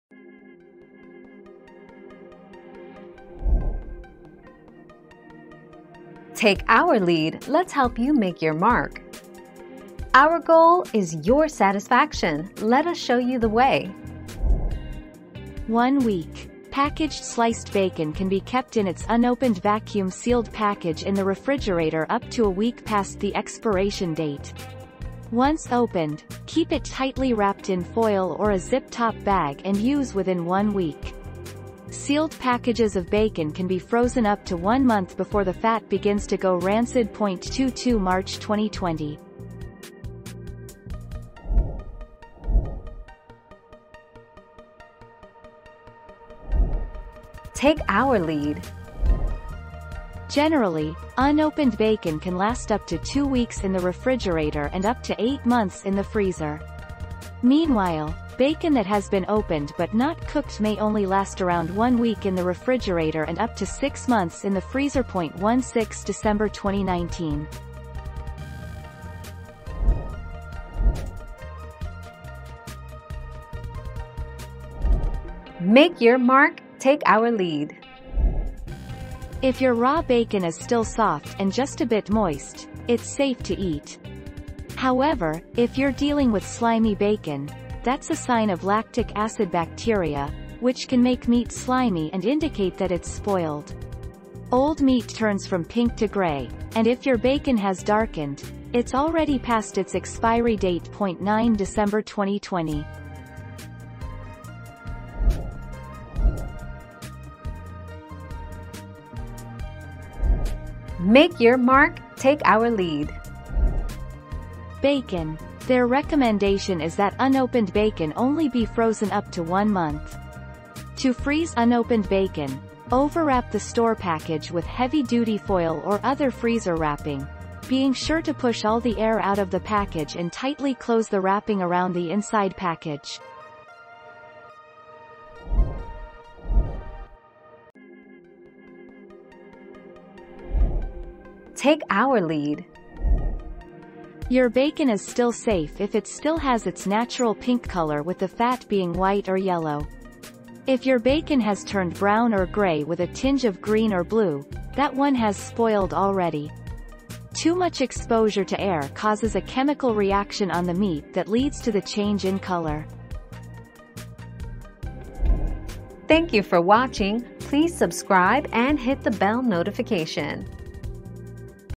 How long can you keep an opened package of bacon?
00:00 - How long can you keep an opened package of bacon?
00:43 - Can Bacon go bad in the fridge?
01:11 - Is slimy bacon OK?
01:46 - Can u freeze bacon?
02:16 - Is Brown bacon okay to eat?

Laura S. Harris (2021, March 13.) How long can you keep an opened package of bacon?
    AskAbout.video/articles/How-long-can-you-keep-an-opened-package-of-bacon-229115